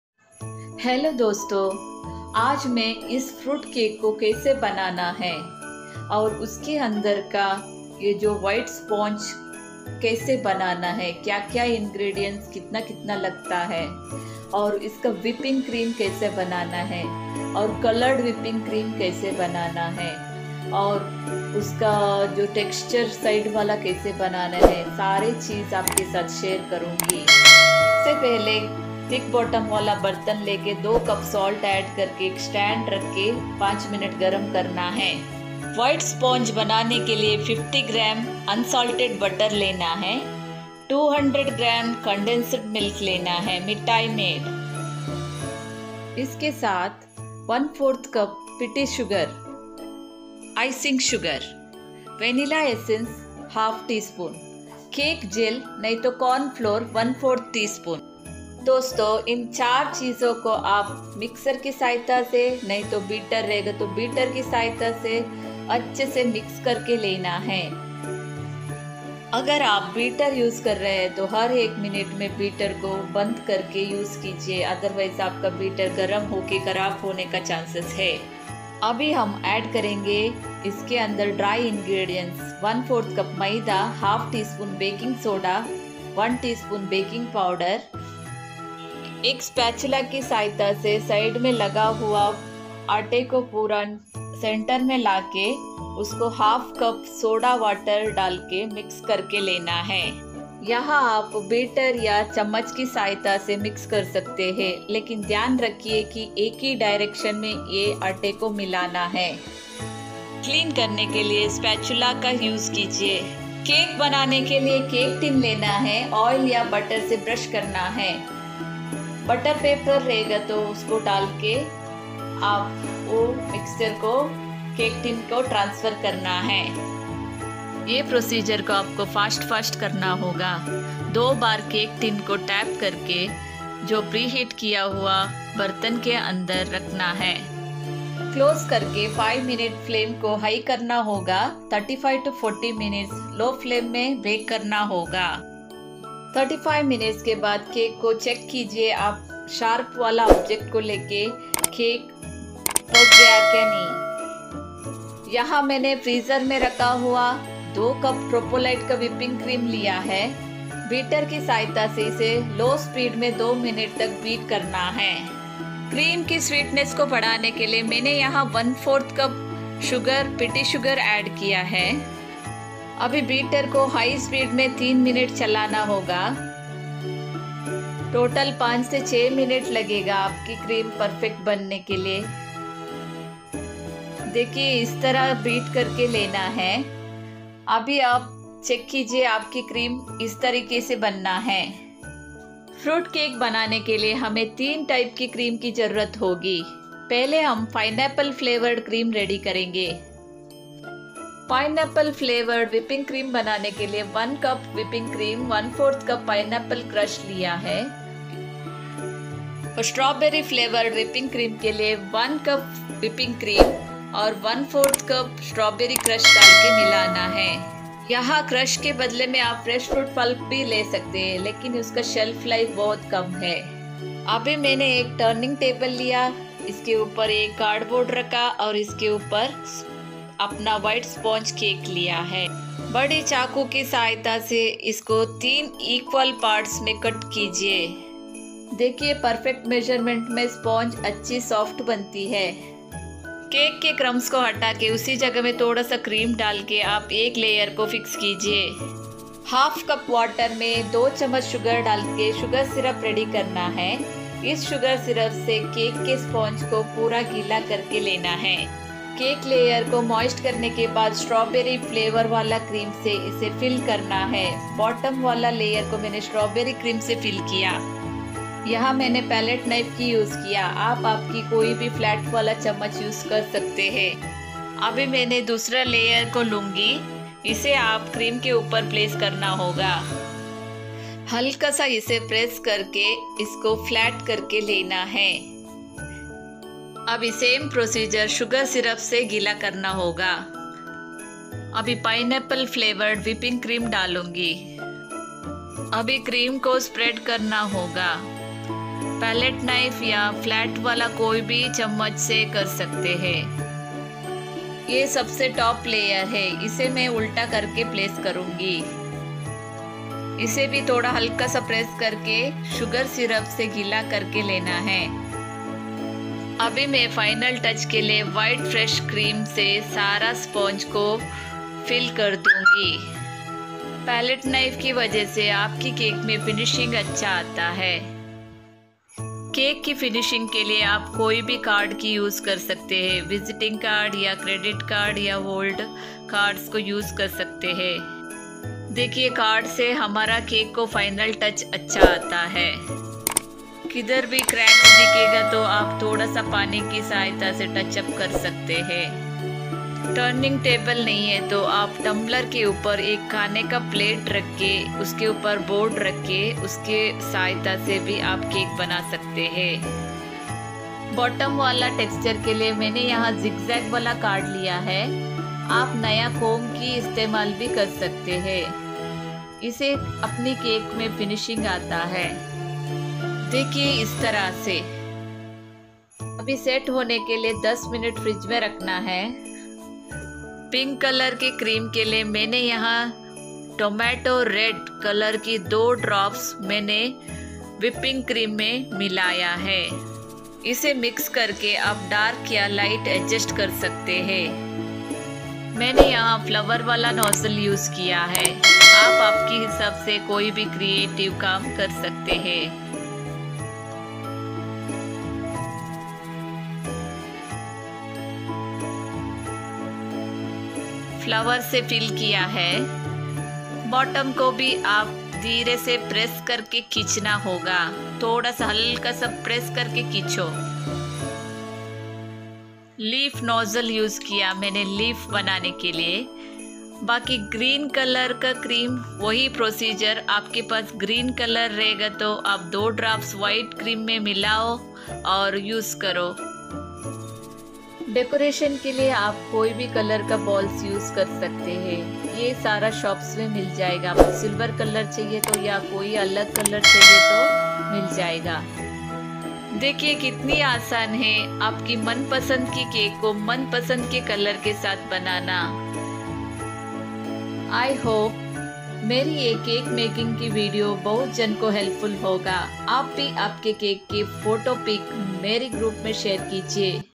How to make Fruit Cake | BEST CAKE COURSE FOR BEGINNERS|WHITE SPONGE MAKING | WHIPPING CREAM
FRUIT CAKE

Make white sponge without egg,
Make whipping cream...
Make coloured whipping cream...
Information on Simple tools we use to make cake..

INGREDIENTS

1 1/4cup maida
1/2cup strawberry crush
200g condensed milk
1/2cup powdered sugar
cream for whipping (tropgolite) 2cups
50g butter
1/2tsp baking soda
1tsp baking powder
1/2tsp vanilla essence
1/4tsp cake jel(cornflour)

INSTRUCTIONS

BAKING IN KADAI: Add 1 cup salt inside the kadai. Use unused old kadai for this preparation.Keep rack or small stand refer video... Heat kadai with closed lid for 10minutes before placing cake tin.

EGGLESS WHITE SPONGE
In a deep mixing bowl add butter, condensed milk, essence, cake jel, powdered sugar beet... or mix well.
Add maida,  baking powder, baking soda.
Mix together.Pour this to butter greased tin.. refer video... place tin inside.. first 3minute bake in high flame... keep flame in low thereafter till 35 to 40 minutes.
Check done or not using toothpick...

WHIPPING CREAM :In a deep plastic container add cream and 1/4cup powdered sugar...beet first 2 minutes in low speed, next 3 minute in medium and high speed... For procedure follow the video.

you can use fresh fruit pieces or fruit crush for fruit flavour.. i used strawberry crush with whipped cream. Mix 1/4cup crush in 1 cup whipped cream... you can use pineapple crush too for a change. diffrent flavours you can use to make one cake also..

Cake filling or decorating :
Make three slices of cake... pour sugar syrup.Moist sponge nicely...spread the crush added cream inside.. same procedure for next layer follow.. cover whole cake with plain whipped cream...  add jel colour or water color to change colour of cream... refer video..
Decoration use star nozzle or dress cake using your imagination...